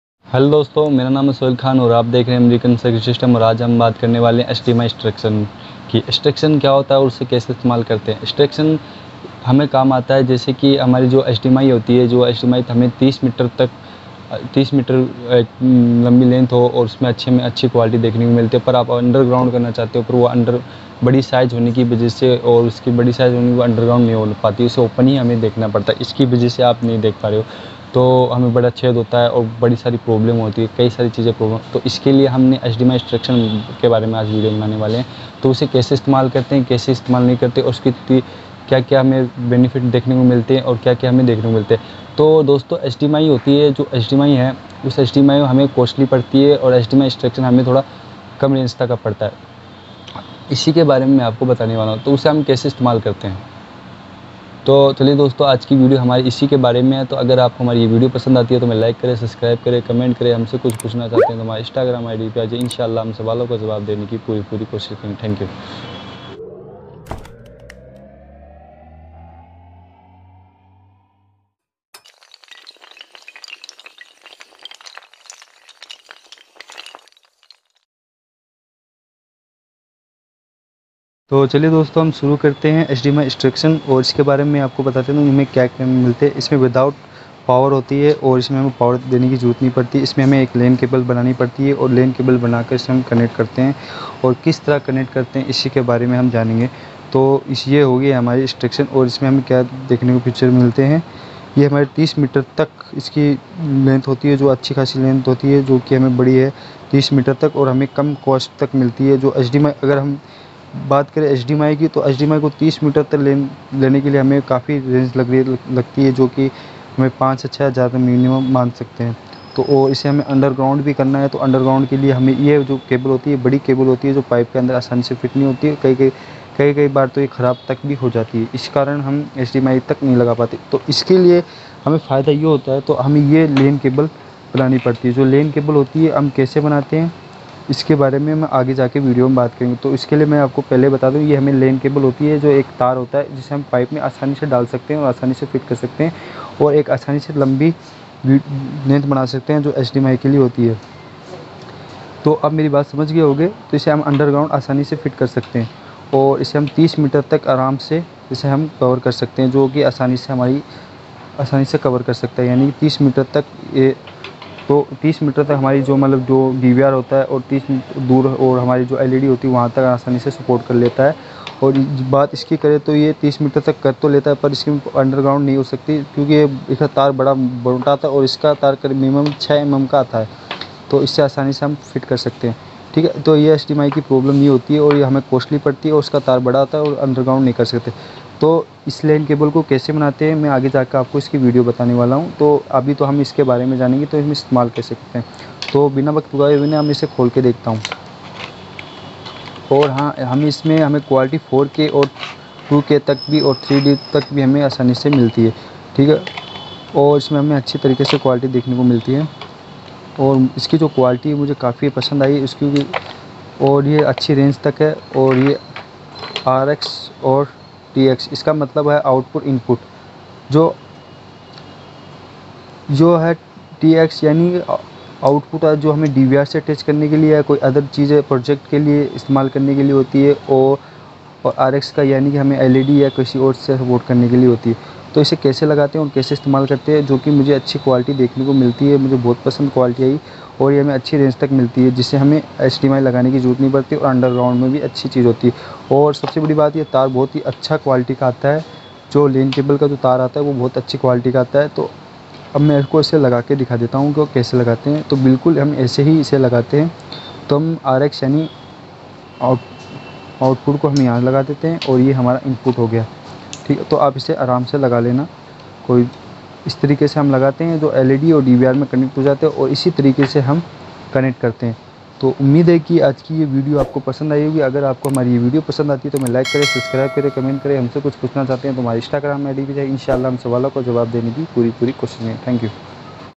HDMI experience | How to make HDMI experience | American security system
About:-
Hello dosto aap ya par CCTV camera IP camera aur amazon ke padka milte rhe ge aur bi CCTV camera ke life updated dete rehege

Hello dosto Aaj ham aapko ye bata ne wale hai ki HDMI experience kya HDMI ka iest mal kese kar te hai aur  ki tar iest ma kar te hai